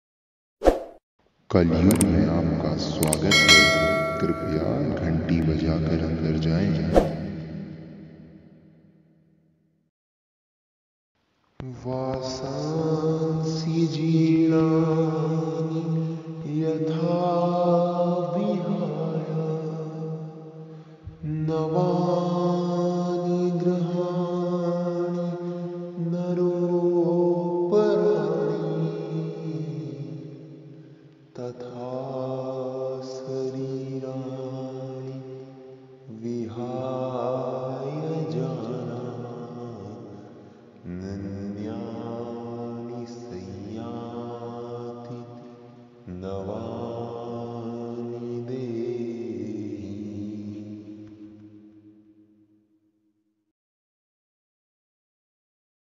श्रीमद्भागवत गीता | geeta shlok | गीता श्लोक | bhagwat gita shlok | sri krishna
A famous shlok from geeta , lord krishna teaches to Arjuna that soul is real everything else is just an illusion
The soul changes body as we changes our clothes

वासांसि जीर्णानि यथा विहाय
नवानि गृह्णाति नरोऽपराणि ।
तथा शरीराणि विहाय जीर्णा-
न्यन्यानि संयाति नवानि देही ।।२२।।

जैसे मनुष्य पुराने वस्त्रोंको त्यागकर दूसरे नये वस्त्रोंको ग्रहण करता है, वैसे ही जीवात्मा पुराने शरीरोंको त्यागकर दूसरे नये शरीरोंको प्राप्त होता है । २२

This shloka belongs to bhagwat gita , at the time of mahabharata bhagavan sri krishna teaches so much things to arjun (parth) . This is one of them
Jay sri krishna